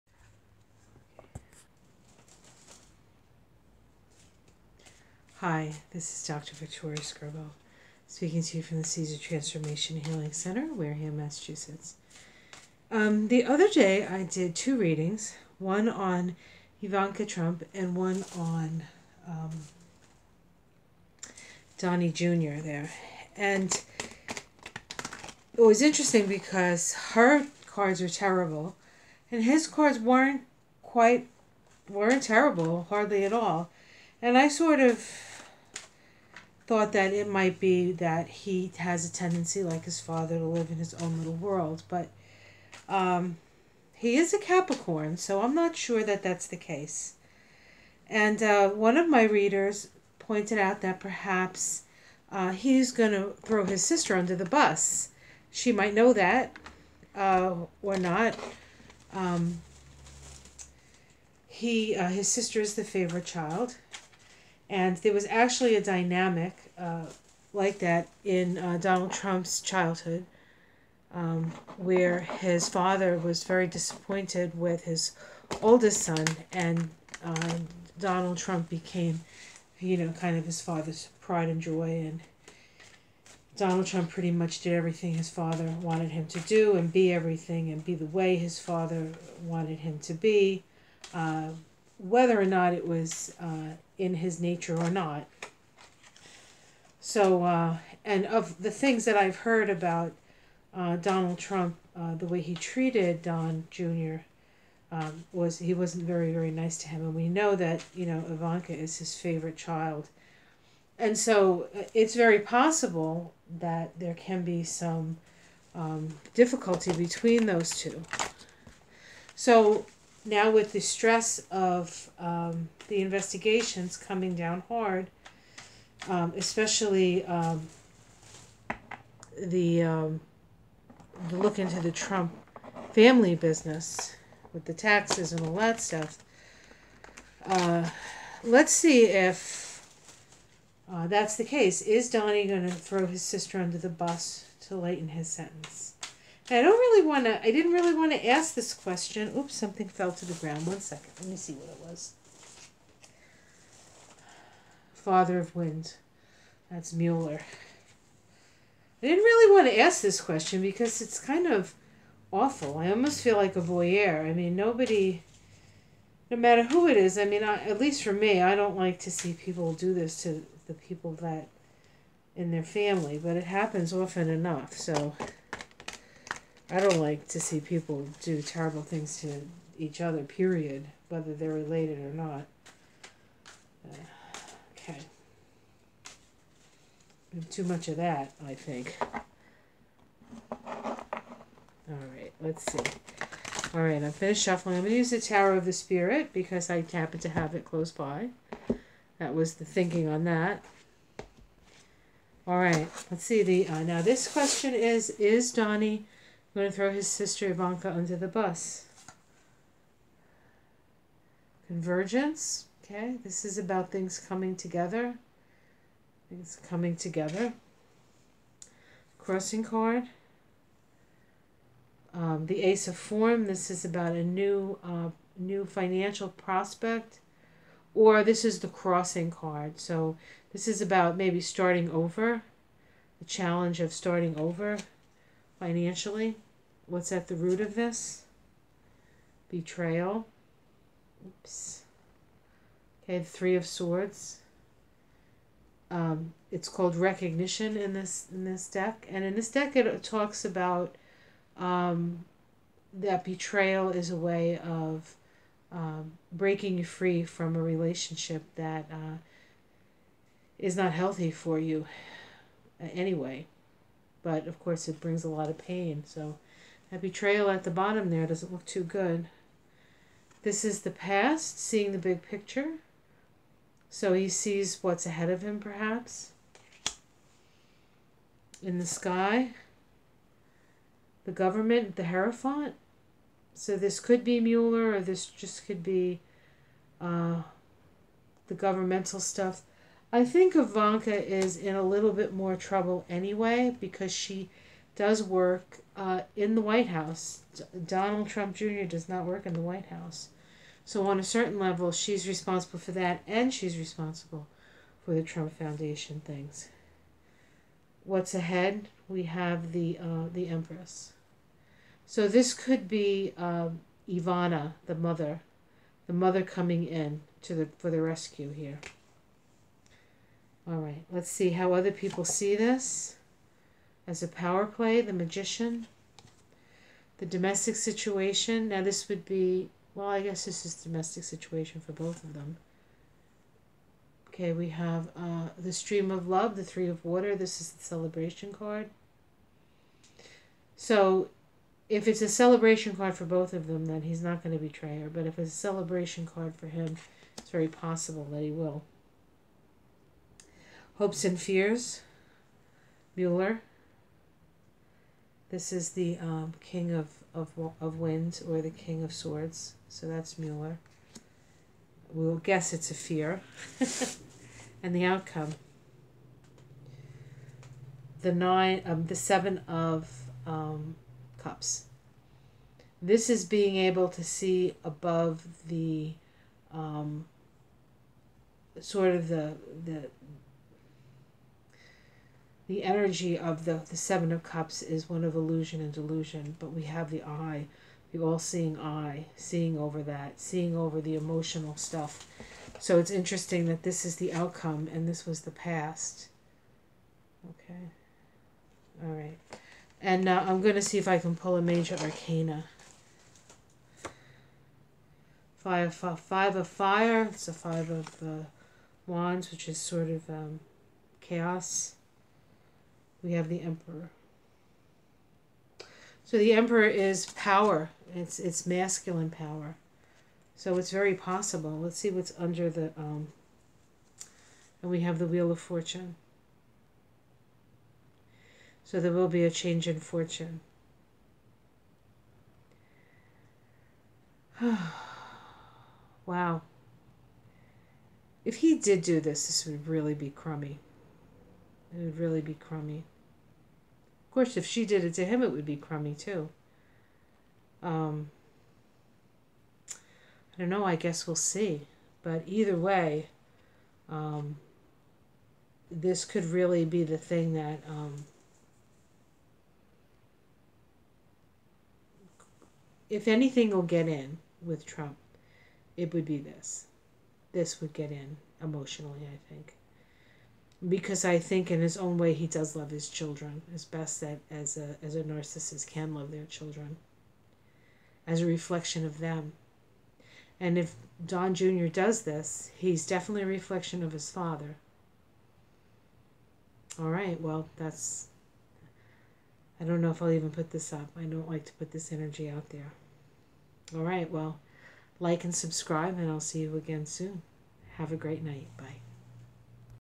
Will Don Jr. betray his sister Ivanka for a lighter sentence?
After a comment by a viewer this question kept nagging at me. Let's see what the cards have to say. Reading done on December 3, 2018.